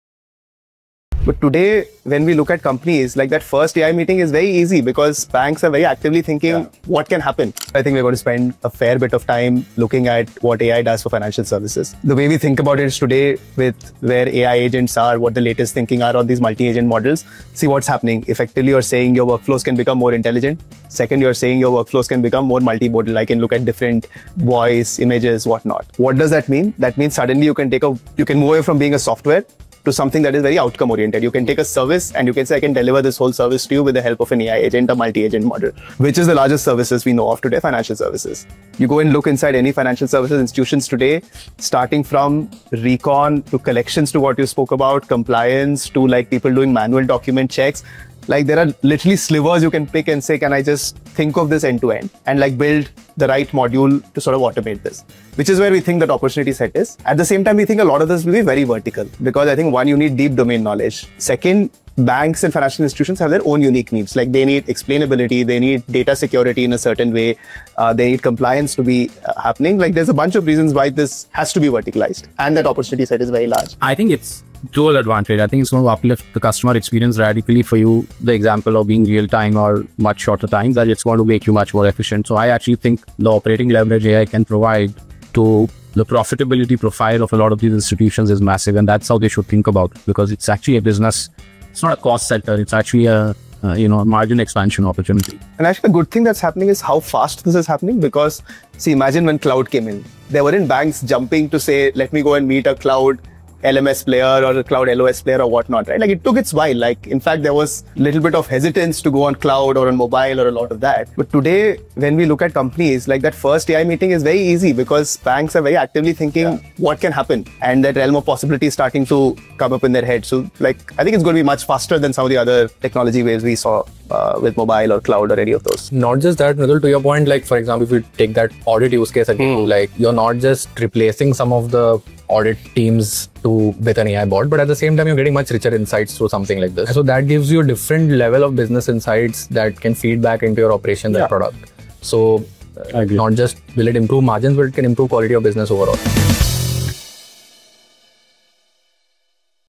The AI Advantage In Financial Services
“AI in financial services is not a cost center, it’s a margin expansion opportunity…”

Financial institutions in India are eagerly adopting AI use cases across their operations — from compliance and collections to routine document checks and audits. This is giving rise to massive opportunities for founders who can deliver verticalized solutions that address the unique needs of this industry.

Vaas Bhaskar, Mridul Arora and Kshitij Jayakrishnan break down why the impact of AI in financial services will be faster and deeper than previous tech waves.